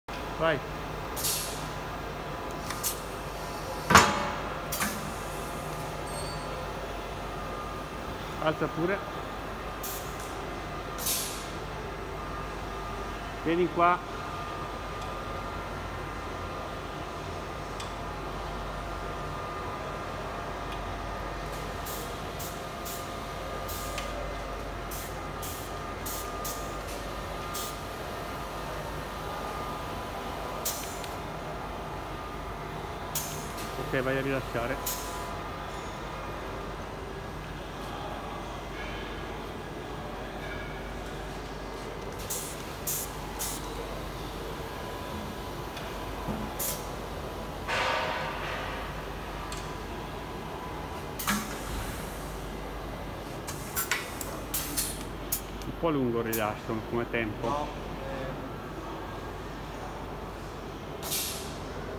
FAMATEC MAGNETO Movimentazizone LAMIERE
FAMATEC MAGNETO per movimentare Lamiere  lamiere di grosso spessore  con i più alti standard di sicurezza, efficienza e affidabilità